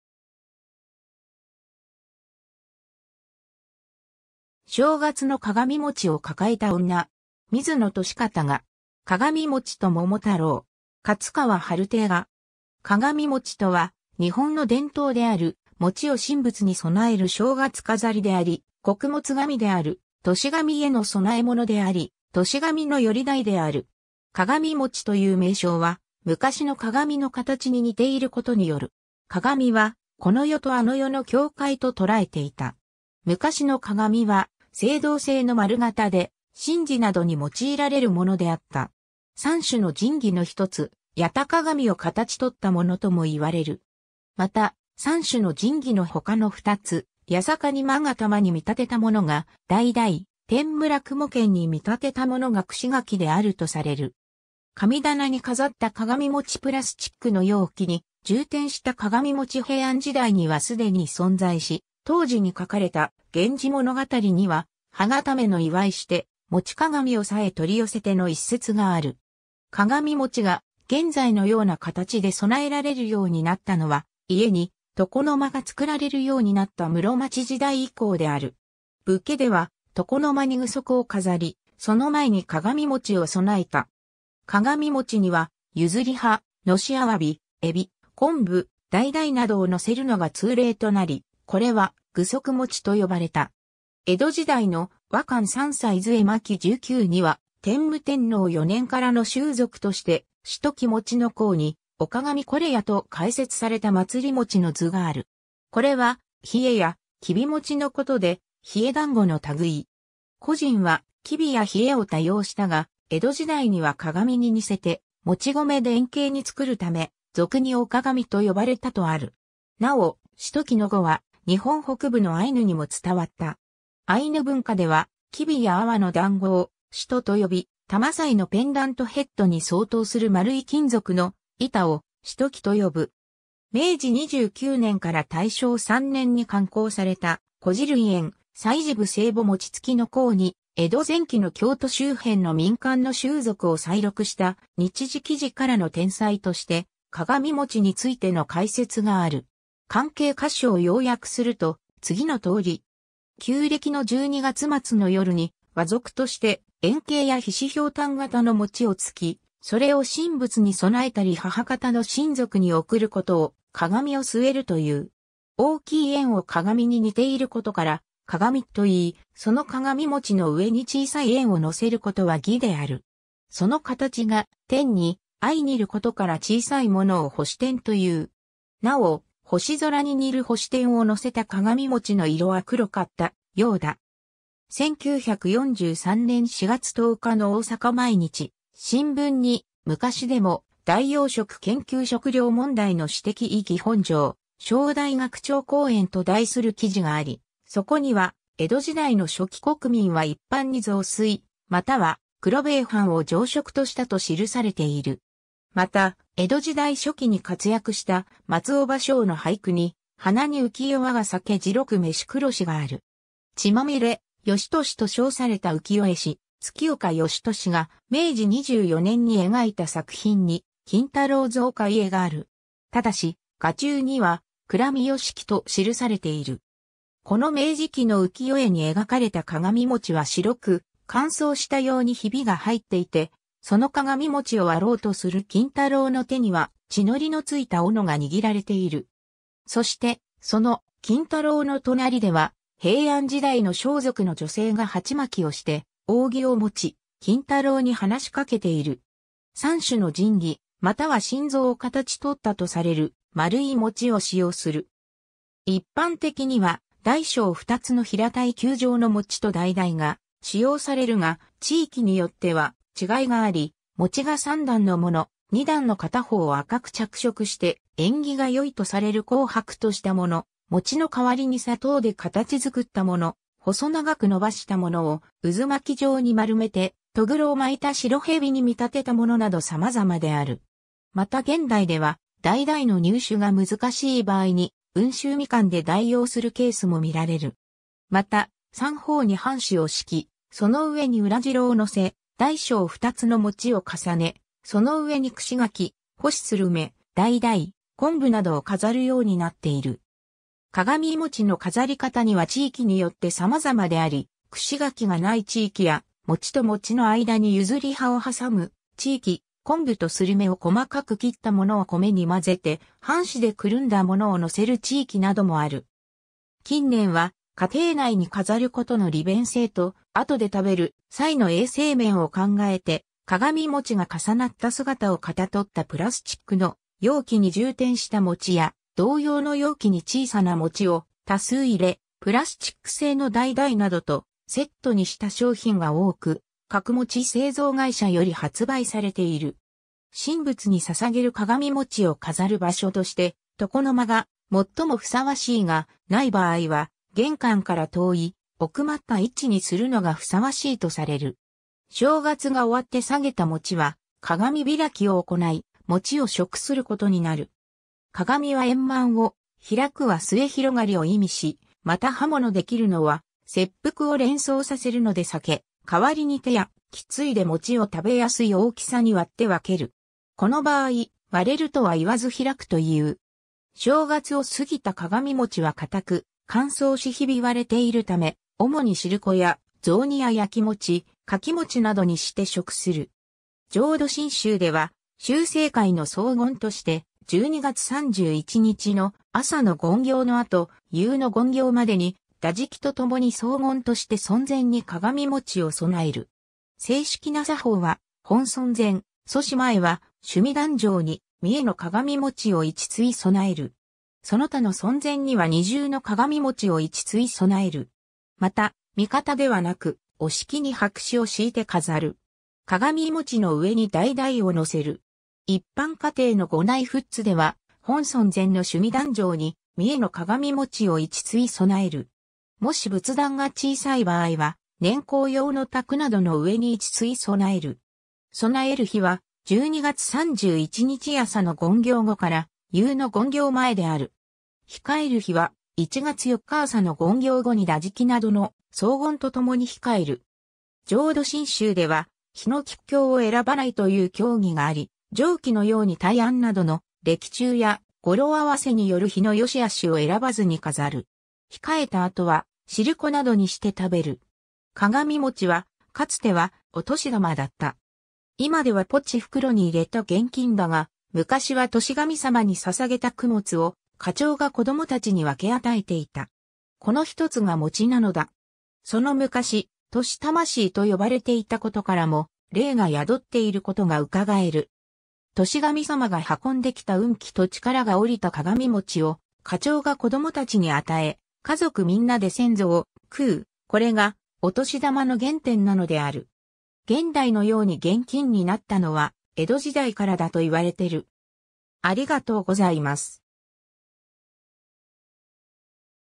鏡餅
正月の鏡餅を抱えた女。
水野年方画。
 鏡餅と桃太郎。
勝川春亭画。
 鏡餅（かがみもち）とは、日本の伝統である、餅を神仏に供える正月飾り（床飾り）であり、 穀物神である「年神（歳神）」への供え物であり、「年神（歳神）」の依り代である。
 鏡餅という名称は、昔の鏡の形に似ていることによる。
鏡はこの世とあの世の境界と捉えていた。
昔の鏡は青銅製の丸形で、神事などに用いられるものであった。
三種の神器の一つ、八咫鏡を形取ったものとも言われる。
また、三種の神器の他の二つ、八尺瓊勾玉に見立てた物が橙（ダイダイ）、天叢雲剣に見立てた物が串柿であるとされる。
 神棚に飾った鏡餅 プラスチックの容器に充填した鏡餅 平安時代にはすでに存在し、当時に書かれた『源氏物語』には「歯固めの祝ひして、餅鏡をさへ取り寄せて」の一節がある。
 鏡餅が現在のような形で供えられるようになったのは、家に床の間が作られるようになった室町時代以降である。
 武家では、床の間に具足（甲冑）を飾り、その前に鏡餅を供えた。
鏡餅には、譲葉・熨斗鮑・海老・昆布・橙などを載せるのが通例となり、これは具足餅（武家餅）と呼ばれた。
 江戸時代の『和漢三才図会』巻19（正徳2年、1712年成立）には、天武天皇4年（675年）からの習俗として、「しとき餅」の項に「御鏡是也」と解説された祭餅の図がある。
これは、稗や黍餅のことで、稗（きび）団子の類。
古人は黍や稗を多用したが、江戸時代には鏡に似せて糯米で円形に作るため、俗に御鏡と呼ばれたとある。
 なお、「しとき」の語は日本北部のアイヌにも伝わった。
アイヌ文化では、黍や粟の団子を「シト」と呼び、タマサイ（女性用の首飾り）のペンダントヘッドに相当する円い金属の板（円鏡）を「シトキ」と呼ぶ。
 明治29年（1896年）から大正3年（1914年）に刊行された『古事類苑』「歳時部」‐歳暮‐餅搗の項に、江戸前期の京都周辺の民間の習俗を採録した「日次紀事」からの転載として、鏡餅についての解説がある。
 関係箇所を要約すると、次のとおり。
旧暦の12月末の夜に、倭俗として円型や菱瓢箪型の餅を搗き、それを神仏に供えたり母方の親族に贈ることを鏡を据えるという。
大きい円を鏡に似ていることから鏡と言い、その鏡餅の上に小さい円を載せることは義である。
その形が天に相似ることから小さいものを星点という。
 なお、星空に似る星点を乗せた鏡餅の色は黒かったようだ。
1943年4月10日の大阪毎日新聞に、「昔でも代用食研究 食糧問題の史的意義 本庄商大学長講演」と題する記事があり、そこには、江戸時代の初期国民は一般に雑炊または黒米飯を常食としたと記されている。
また、江戸時代初期に活躍した松尾芭蕉の俳句に、「花にうき世我が酒白く飯黒し」がある。
 「血まみれ芳年」と称された浮世絵師月岡芳年が明治24年（1891年）に描いた作品に「金太郎蔵開絵」がある。
ただし、画中には「くら美良喜」と記されている。
この明治期の浮世絵に描かれた鏡餅は白く、乾燥したようにひびが入っていて、その鏡餅を割ろうとする金太郎の手には、血のりの付いた斧が握られている。
そして、その金太郎の隣では、平安時代の装束の女性が鉢巻をして扇を持ち、金太郎に話しかけている。
 三種の神器または心臓を形とったとされる、丸い餅を使用する。
 一般的には、大小2つの平たい球状の餅とダイダイが使用されるが、地域によっては違いがあり、餅が三段のもの、二段の片方を紅く着色して縁起が良いとされる紅白としたもの（石川県で見られる）、餅の替わりに砂糖で形作ったもの、細長く伸ばしたものを渦巻状に丸めてとぐろを巻いた白蛇に見立てたものなど様々である。
また現代ではダイダイの入手が難しい場合にウンシュウミカンで代用するケースも見られる。
 また、三方に半紙を敷き、その上に裏白（羊歯の一種）を載せ、大小2つの餅を重ね、その上に串柿・干しするめ・橙・昆布などを飾るようになっている。
鏡餅の飾り方には地域によって様々であり、串柿が無い地域や、餅と餅の間に譲葉を挟む地域、昆布とスルメを細かく切ったものを米に混ぜて半紙でくるんだ物を乗せる地域などもある。
 近年は、家庭内に飾ることの利便性と、後で食べる際の衛生面を考えて、鏡餅が重なった姿を型取ったプラスチック(ポリエチレン)の容器に充填した餅や、同様の容器に（個別包装された）小さな餅を多数入れ、プラスチック製の橙などとセットにした商品が多く、各餅製造会社より発売されている。
 神仏に...